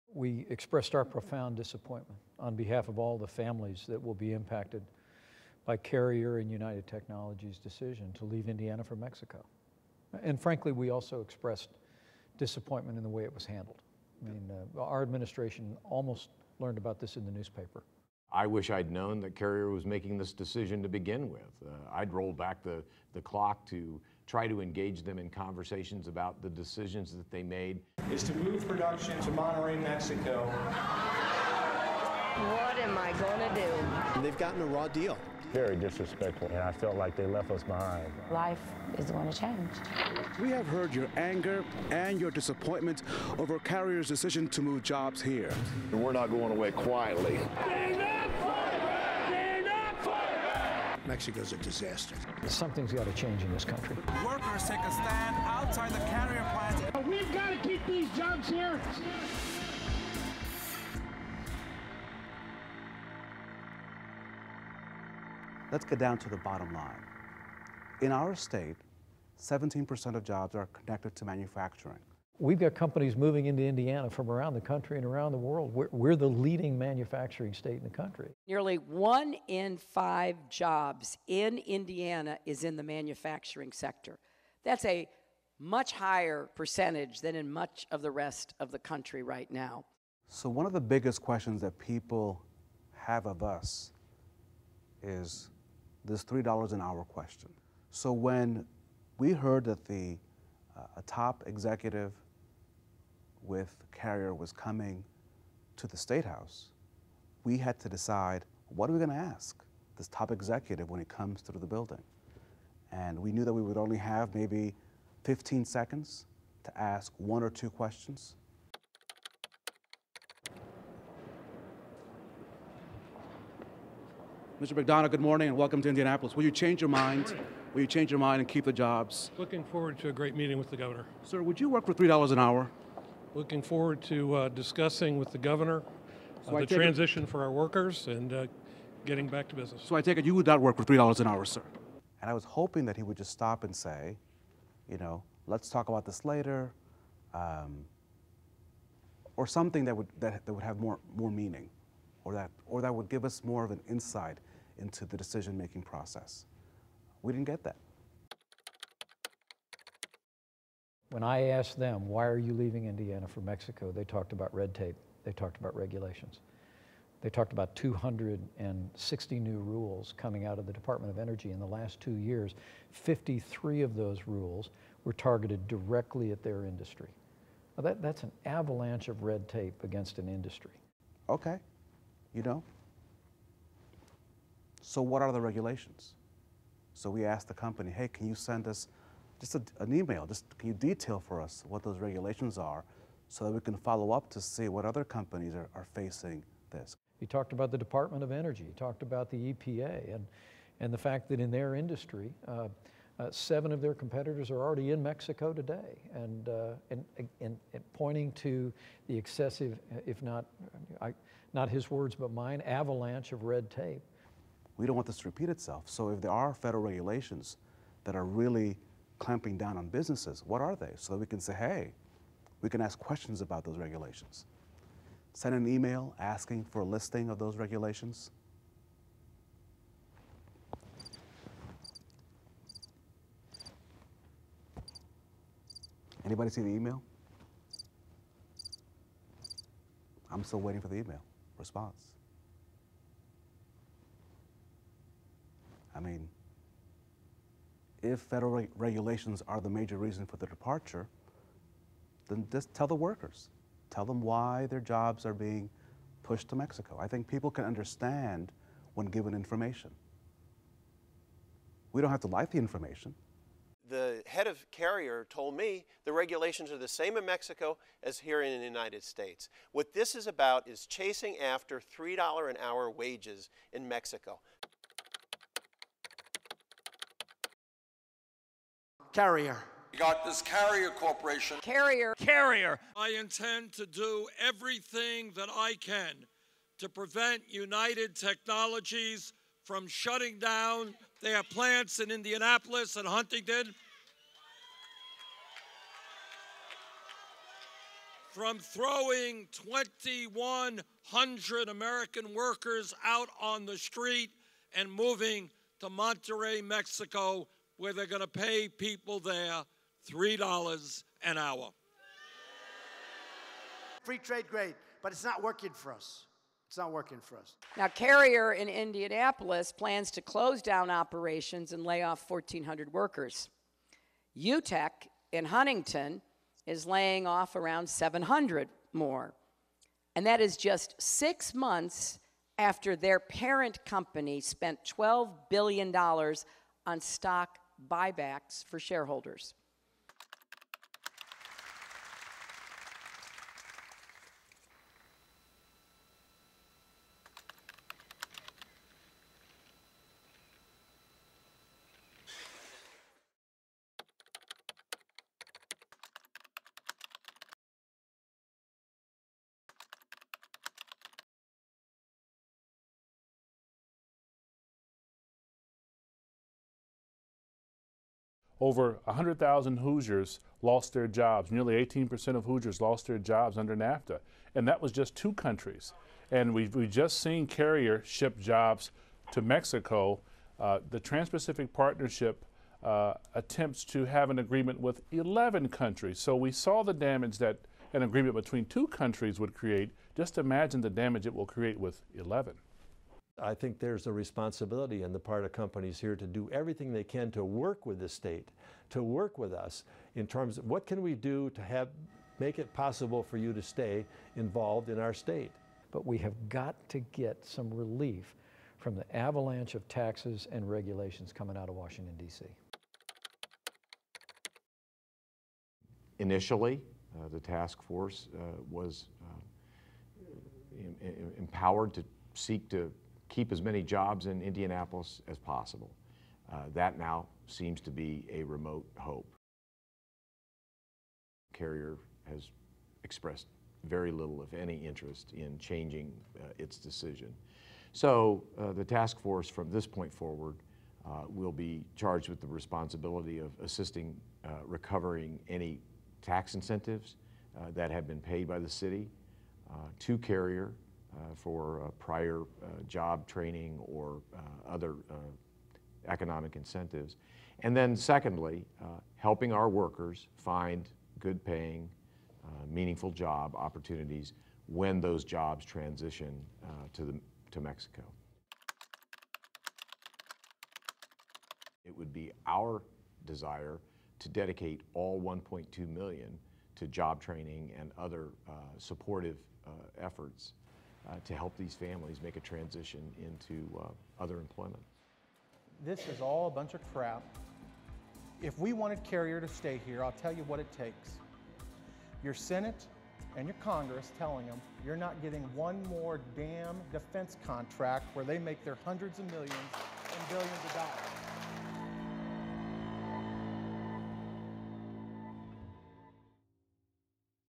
Moving to Mexico Episode 4: Why did Carrier decide to move 1,400 Indy jobs to Monterrey, Mexico?
Who let Carrier take 1,400 Indianapolis jobs to Mexico? How did it happen? Would we have been able to do anything about it if we knew? Exploring these questions with our elected officials and decision makers left frustration in its wake. It's the fourth episode in a series exploring Carrier's decision to move to Monterrey, Mexico.